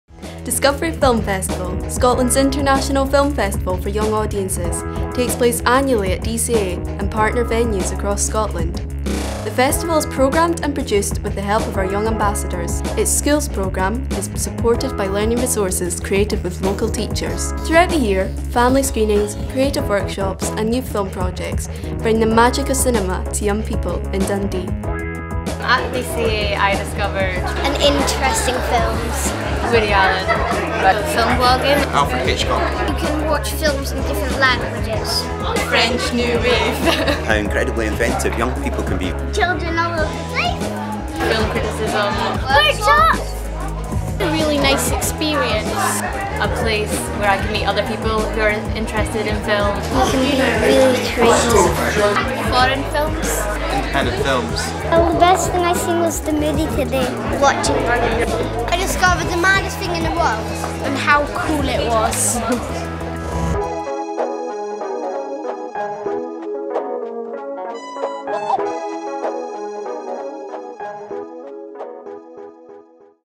DCA Discovery Trailer 2013
Discovery Film Festival 2013: Sat 19 October - Sun 3 November

Scotland's International Film Festival for Young Audiences is back for its tenth year. Join us for another selection of the very best films from around the world, alongside workshops, events, and two special exhibitions in the Galleries.